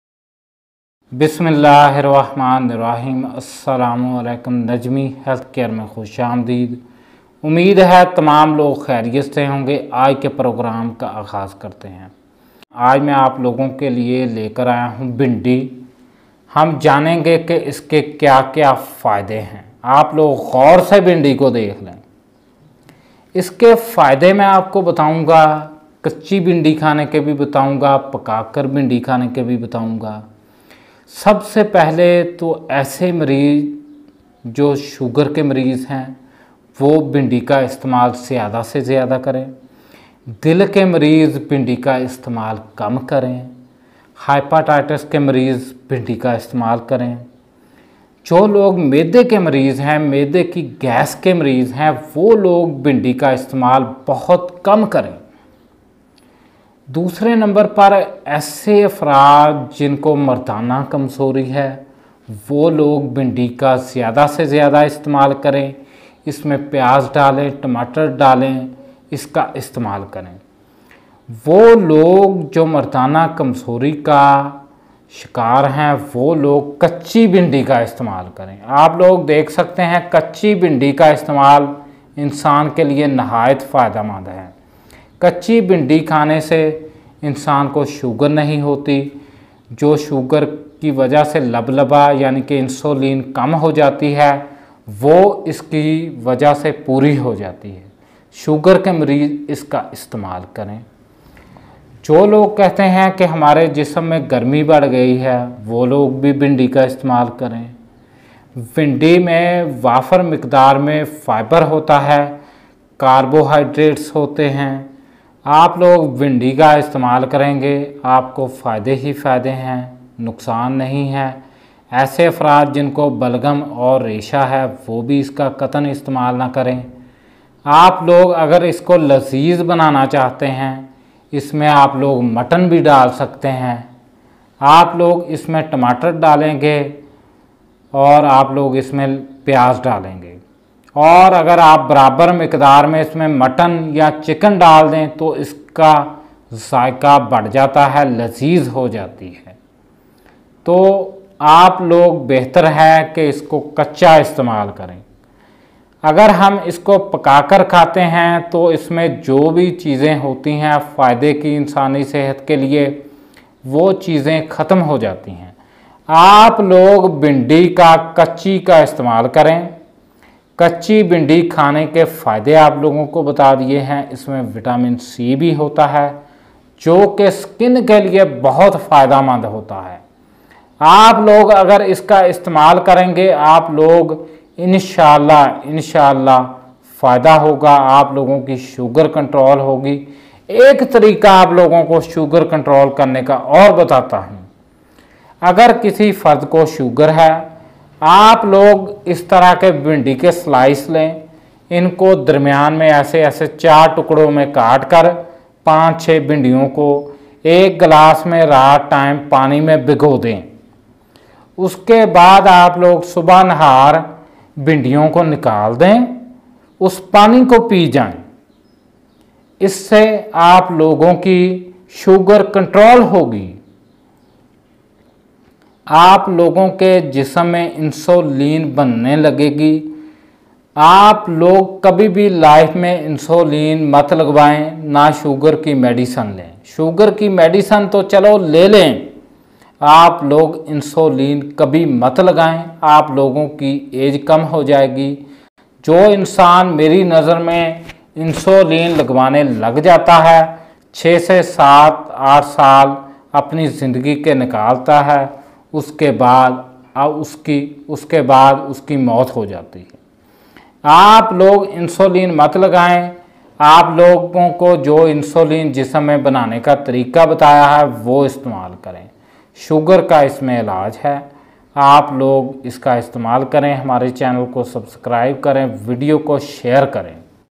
Kachi bhindi khane ke fayde | Controle Blood Sugar Level Naturally | Benefits of Lady Finger Hindi
Today This vedio benefits Lady finger and use sugar pationt controle blood sugar level .